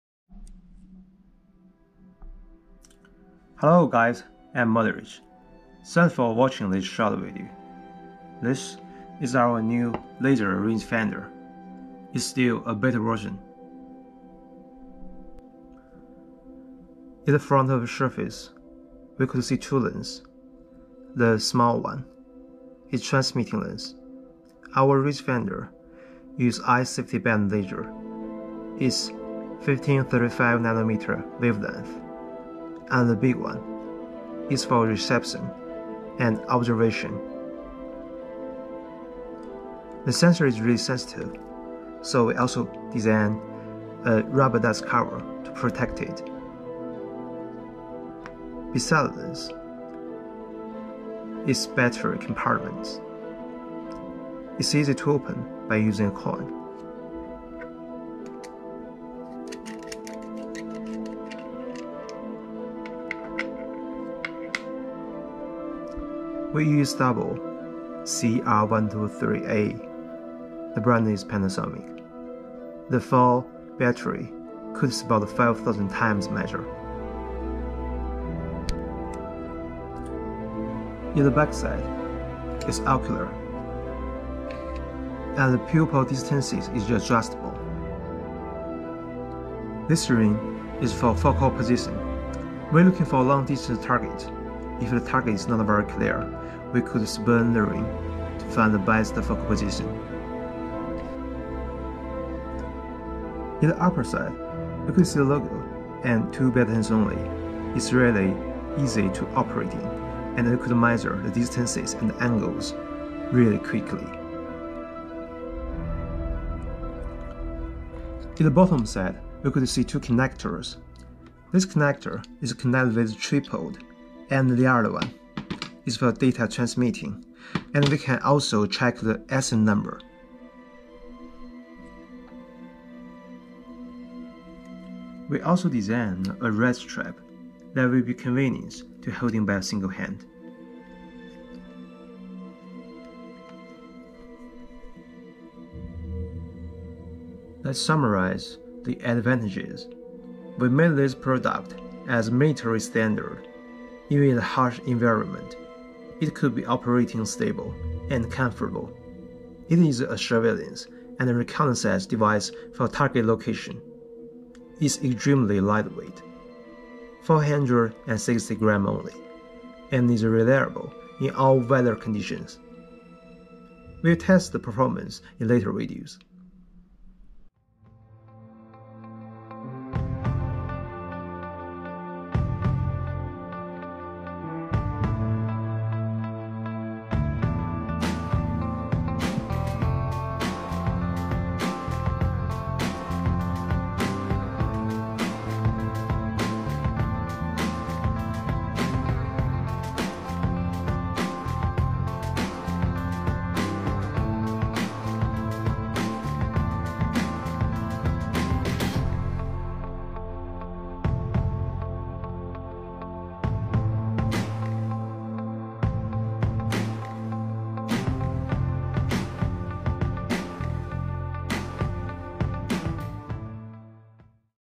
1535nm Eye-safety Laser RangeFinder
We made this product as military standard,even in the harsh environment,it could be operating stable and comfortable.
It is a surveillance and reconnaissance device for target location.it’s extremely lightweight and is reliable in all weather conditions.